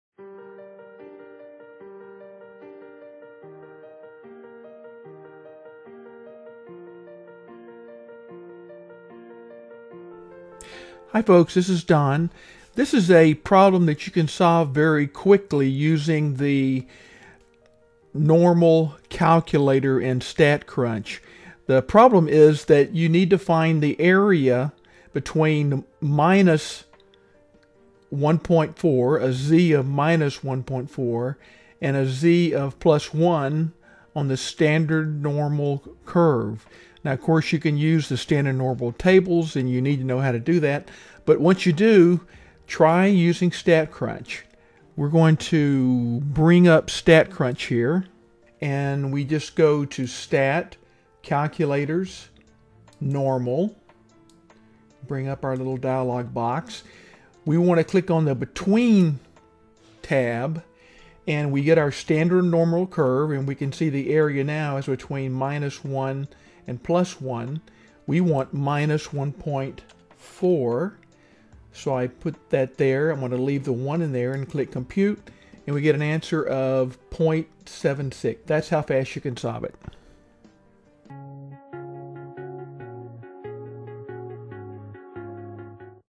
5.1.24 Find the area under the normal curve /probability between two values of z using StatCrunch
A quick way to find the area under the normal curve between two z-scores using the StatCrunch Normal Calculator. Larson MyStatLab Problem 5.1.24